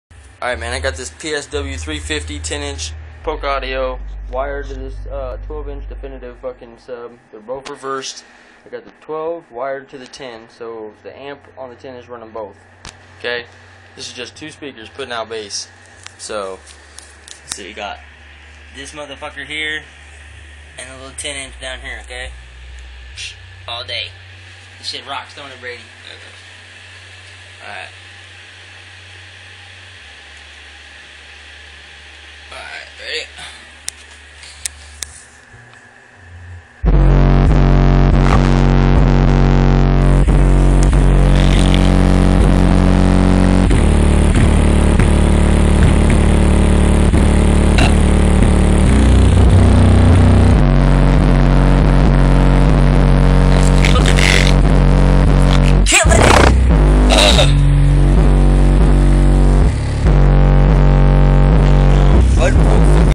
Holy shit listen to the subs
PSW 350 Polk Audio 10 inch reversed sub that I did myself along with a 12 inch definitive reversed to put in a bigger box that is wired to no other speakers in the room and putting off bass just these